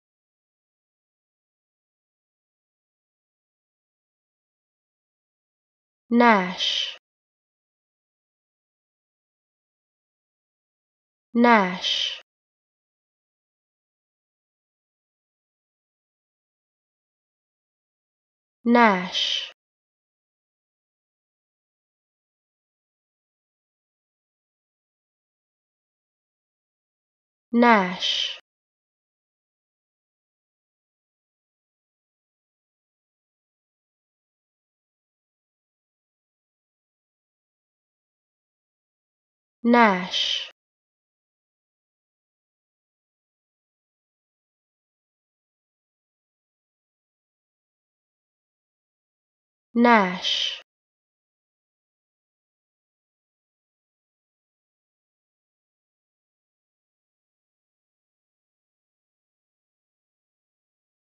How To Say Nash
Learn how to say Nash with EmmaSaying free pronunciation tutorials.